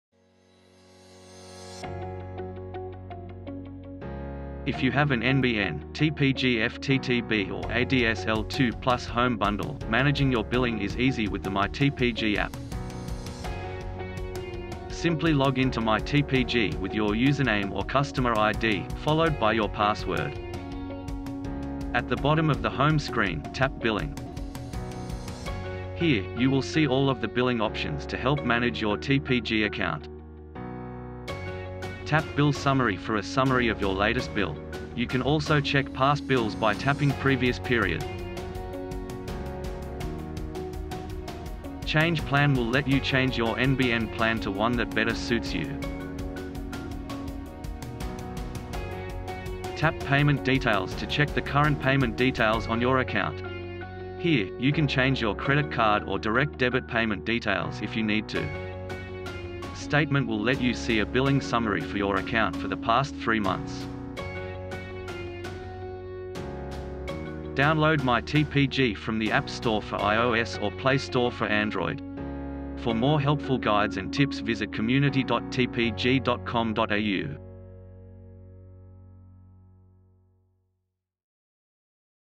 TPG - Manage Your Billing via My TPG
A video guide on how to managing your billing via the My TPG app.